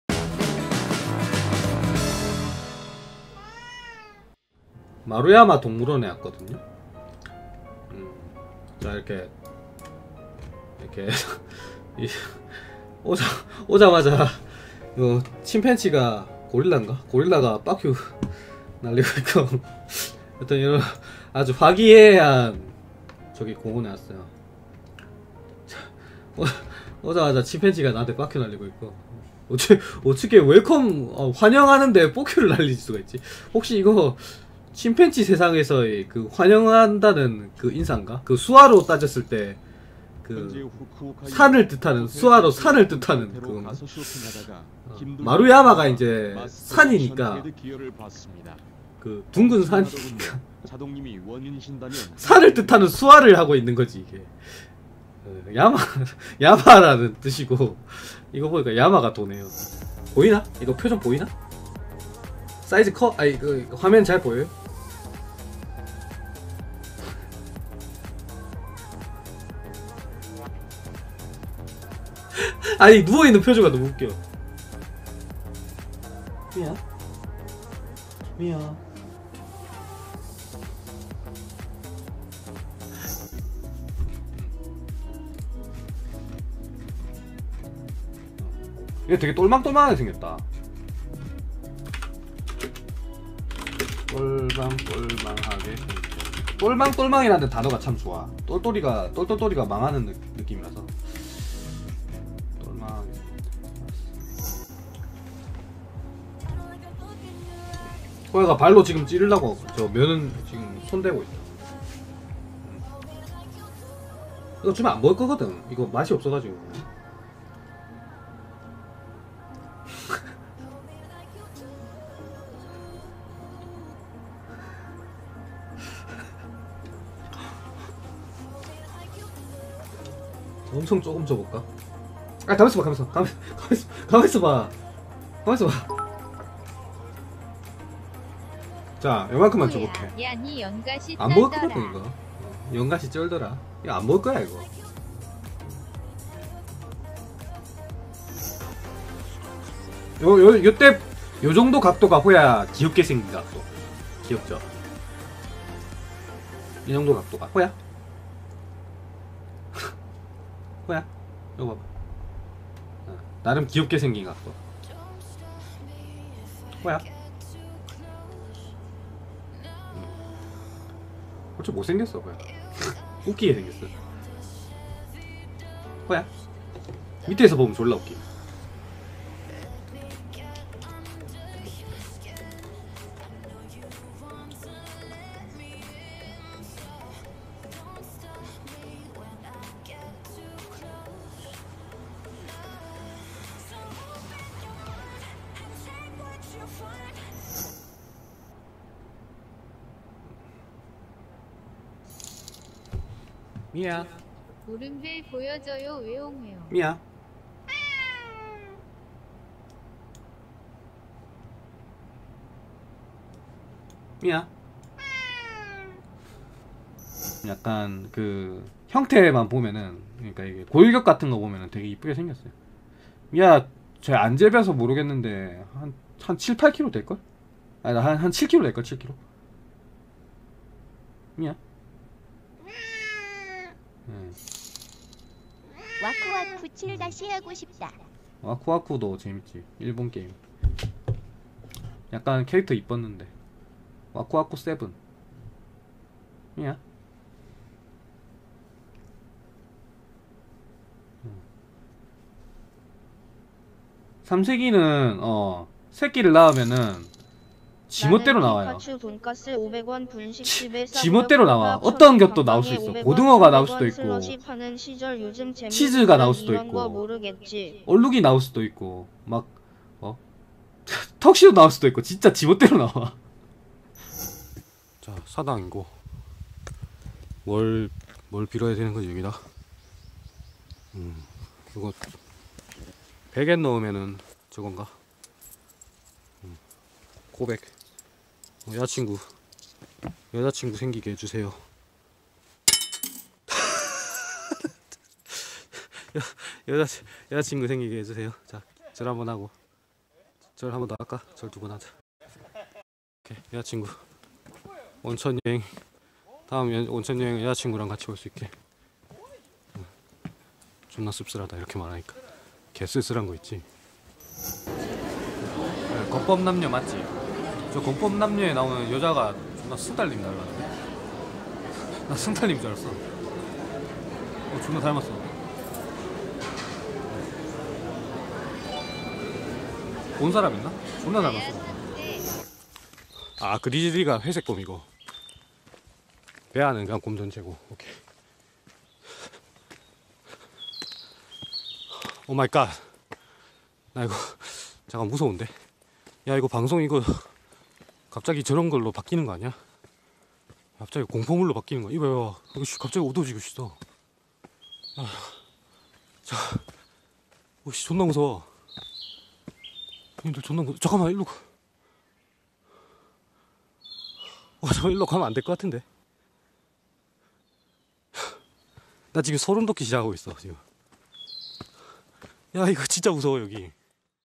[월간 자동 2018 7월호] 침팬지 세계에서 이 동작은 환영을 뜻합니다 / 7월 핫클립 하이라이트
+ 7월 한달간 가장 재밌었던 클립들을 꾹꾹 눌러 담았습니다! +
+ 생방송을 보고싶다면 트위치로!